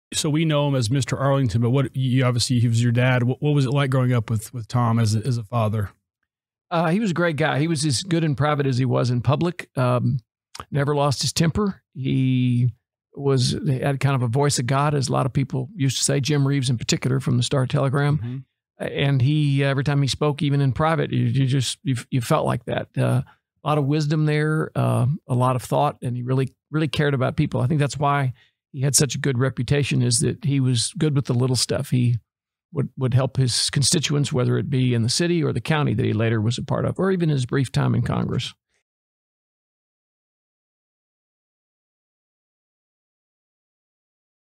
What Was Tom Vandergriff Like as a Father Victor Vandergriff FORTitude FW Podcast Ft Worth
Tom Vandergriff is a legend in North Texas politics, what was he like as a Dad?  We ask Victor Vandergriff and his answer doesn't disappoint.
FORTitude FW - Where the Stories Never Die.
A Fort Worth centric podcast featuring luminaries and everyday Fort Worthians sharing their story.